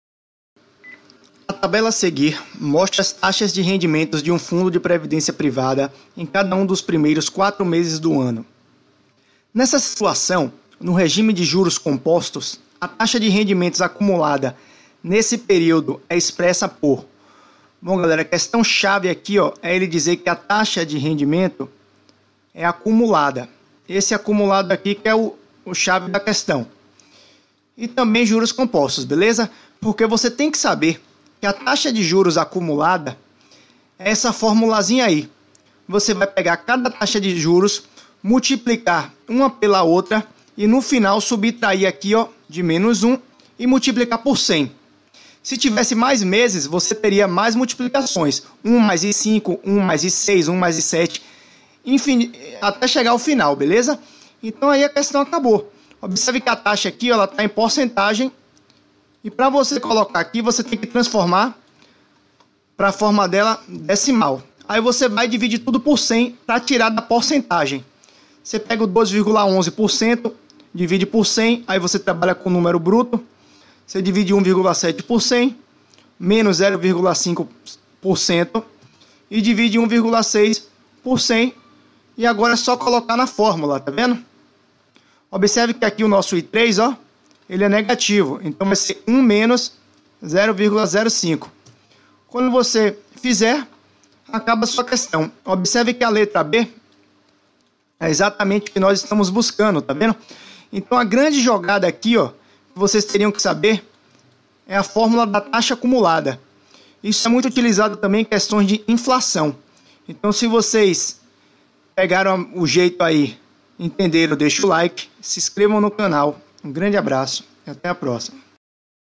CEBRASPE 2019 - SEFAZ RS Auditor Q.83
A tabela a seguir mostra as taxas de rendimentos de um fundo de previdência privada em cada um dos primeiros 4 meses do ano de 201X.
Nessa situação, no regime de juros compostos, a taxa de rendimentos acumulada nesse período é expressa por
A [(2,11 + 1,7 ! 0,5 + 1,6) ! 1] × 100%.
B [(1,0211 × 1,017 × 0,995 × 1,016) ! 1] × 100%.
C [(2,11 × 1,17 × 0,995 ×1,6) ! 1] × 100%.
D (1,0211 + 1,017 ! 1,005 + 1,016)%.
E (2,11 + 1,7 + 0,5 + 1,6)%.